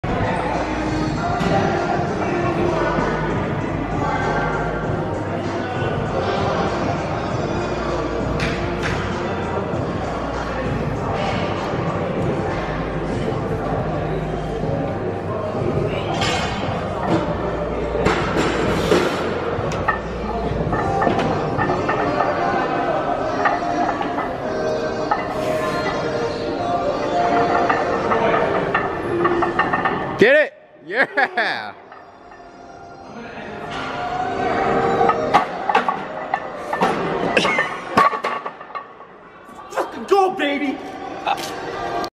NEW SQUAT PR!
370x1 at 173lbs at 19 years of age.